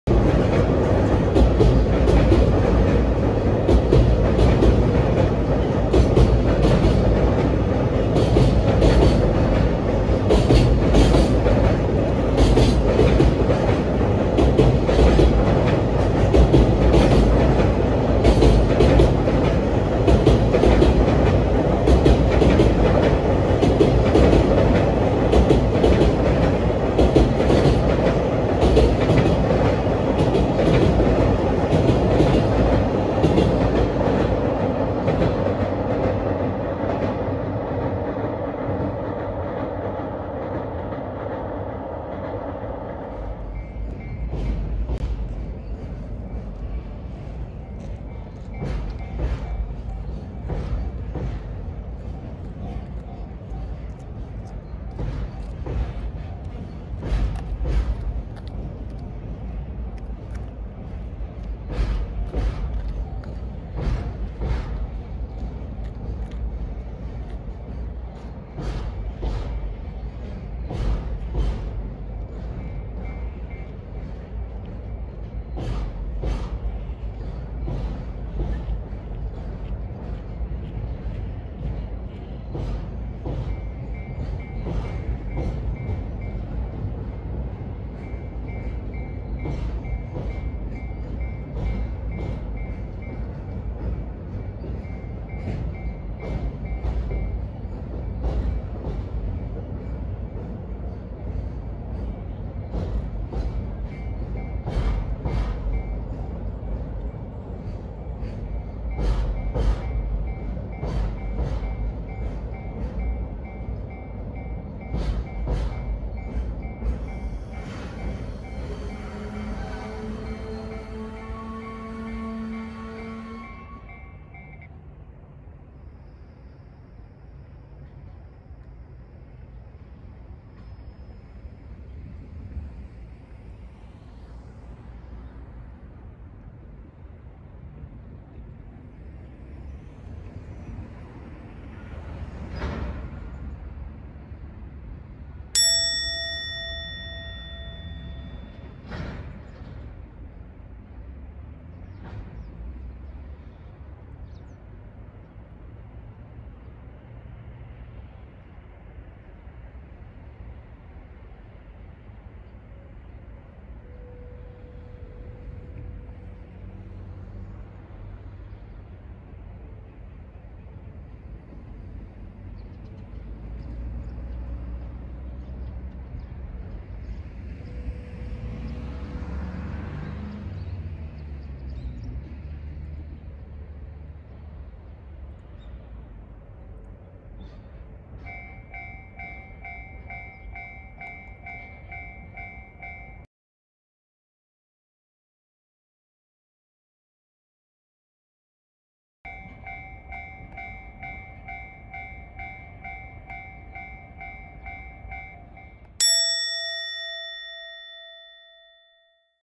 【鉄道ファン】貨物列車がフリーズ/全部見れる方貨物列車ファン確定
旭川橋りょうの上で完全停止
長い間撮影してる中でも珍しいので
記録に残しておきます。

この番組はマニアの中のマニアしか
わからない世界をお届けしてる時も
ありますのでコメントは優しいコメントのみお待ちしております。